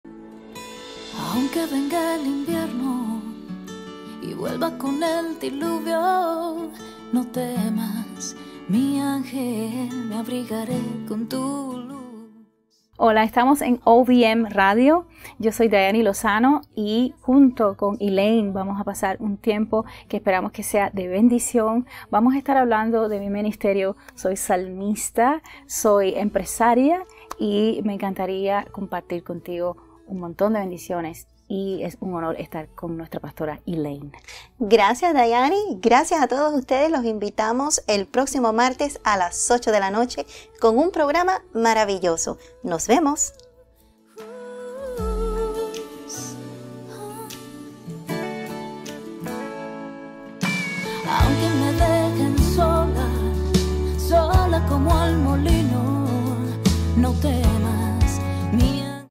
Dayani Lozano, el próximo Martes 17 de noviembre en Compartiendo La Visión - OVM Radio
No te pierdas a Dayani Lozano, consagrada artista catalogada como la pionera de la música pop en Cuba. Estará con nosotros el próximo martes 17 de noviembre a las 8 pm (Miami), en un nuevo Programa de CompartiendoLaVisión con Elaine Enriquez, siempre por OVMRadio.com